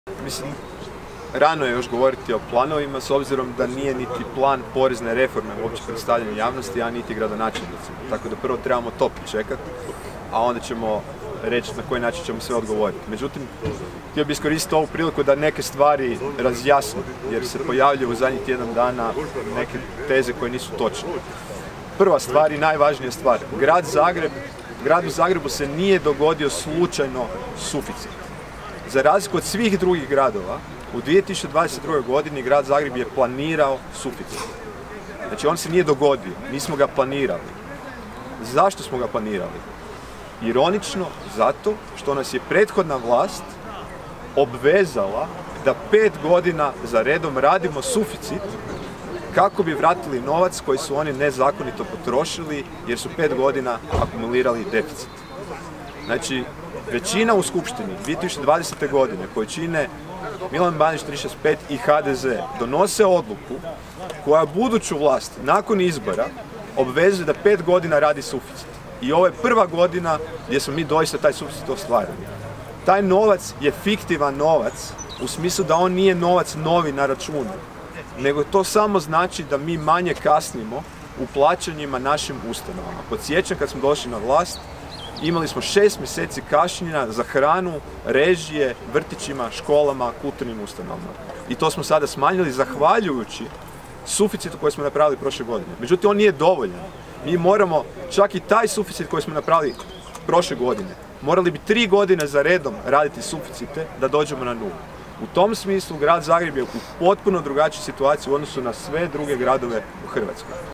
Tomašević: "Zbog Milana Bandića i HDZ-a mi bi 5 godina trebali raditi suficit da vratimo dugove"
Gradonacelnik Zagreba Tomislav Tomašević otkrio je spomen ploču i položio vijenac na spomenik u spomen parku Dotršćina, a nakon toga održao redovnu konferenciju za medije.